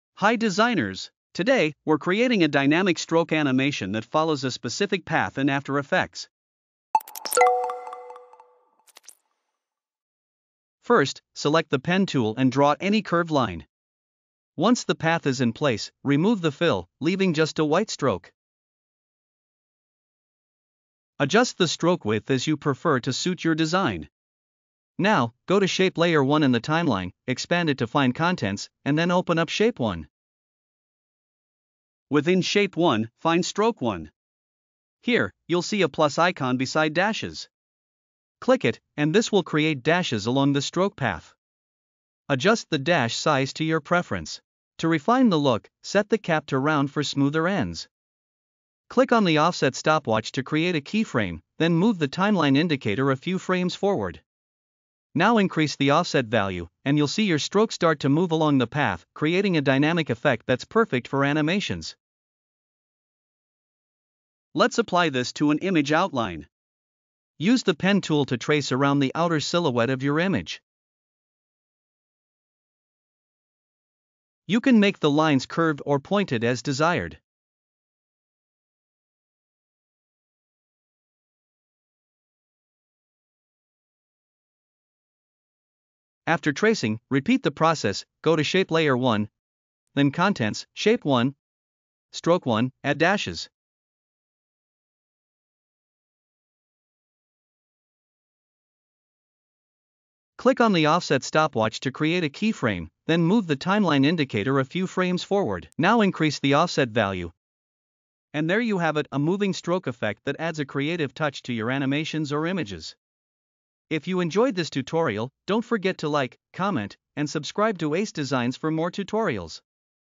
Animating a Moving Stroke Path Effect in After Effects | Step-by-Step Tutorial
🚀 Welcome to Ace Illustration, your destination for creative design and animation tutorials! 🎨✨

🖌️In this tutorial, learn how to create a dynamic stroke animation that follows a path in Adobe After Effects. Using the Pen Tool and simple adjustments to stroke properties, we’ll guide you through animating a line that can enhance your motion graphics and image outlines. Perfect for beginners and pros alike, this easy-to-follow method brings a creative touch to any project.